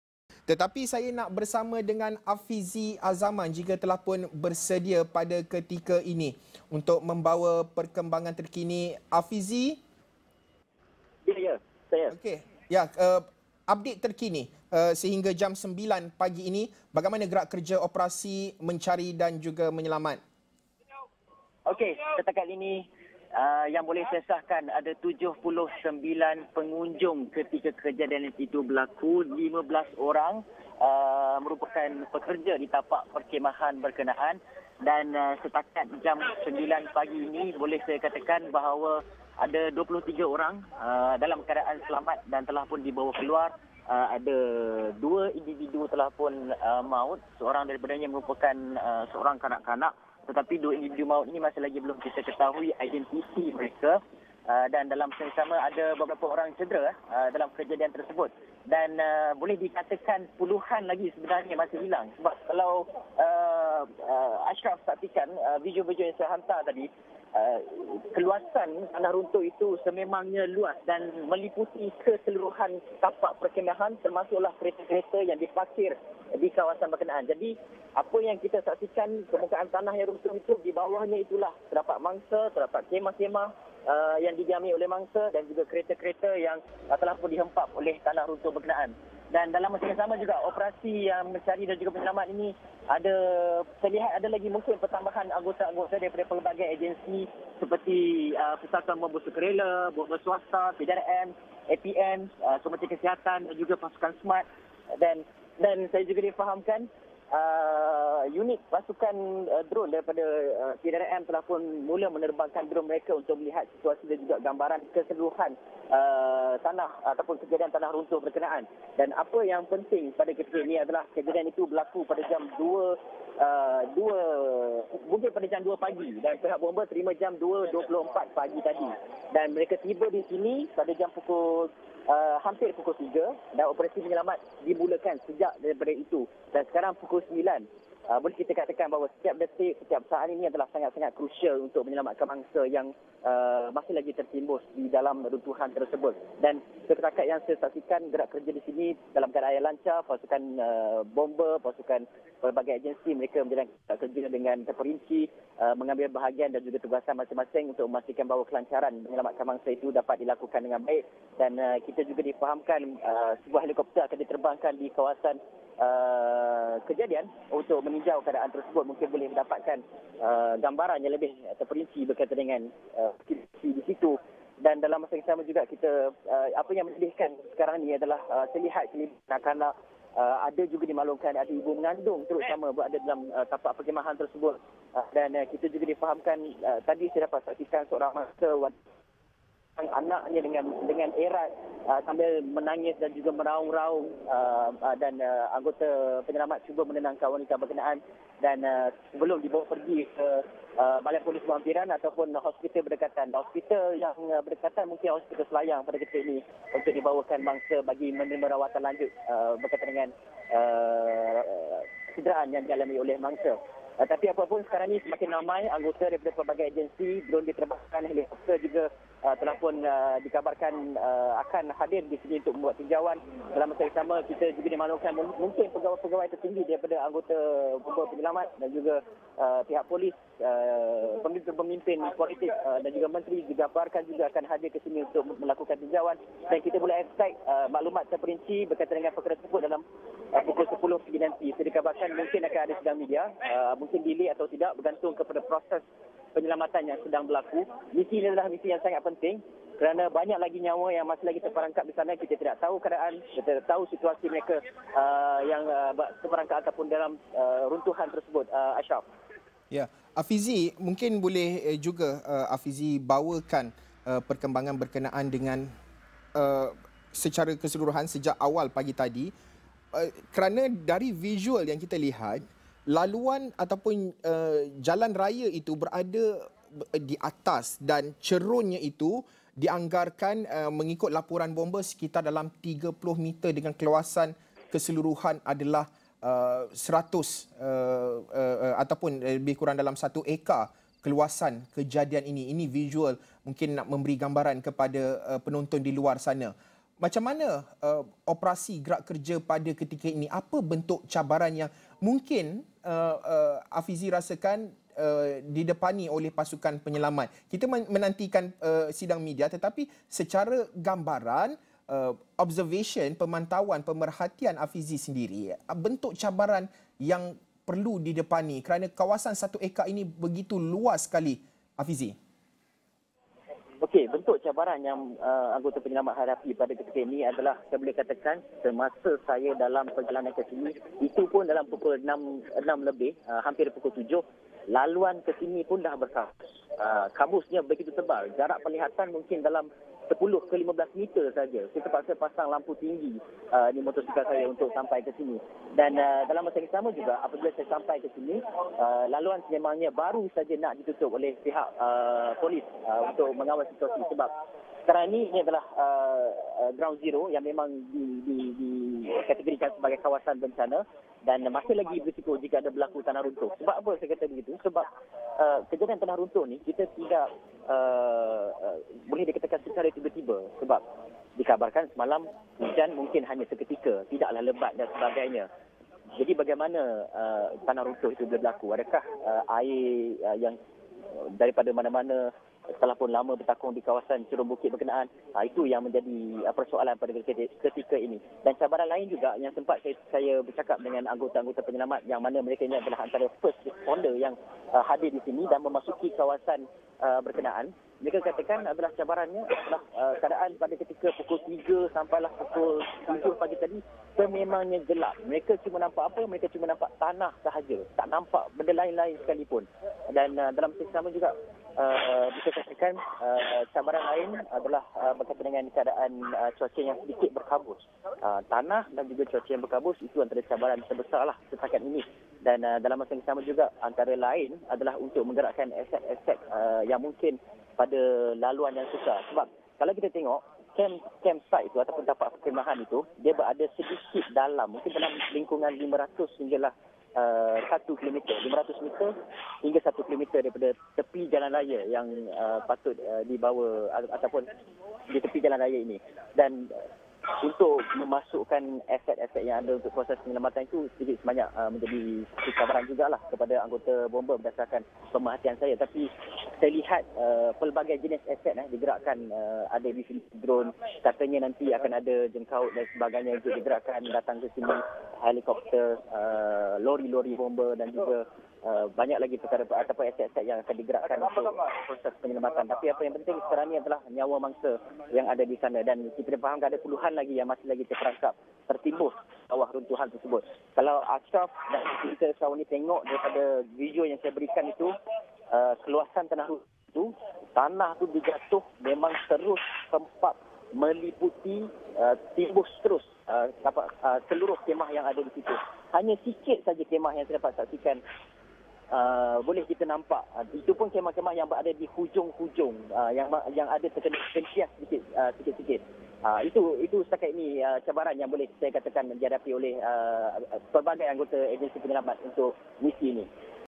Perkembangan tanah runtuh di tapak perkhemahan Batang Kali (Setakat jam 9:30 pagi)
Difahamkan terdapat 79 pengunjung yang berada di tapak perkhemahan itu, manakala 15 pekerja Father Organic Farm. Setakat kini, kira-kira 53 mangsa terselamat, manakala sembilan mangsa disahkan meninggal dunia.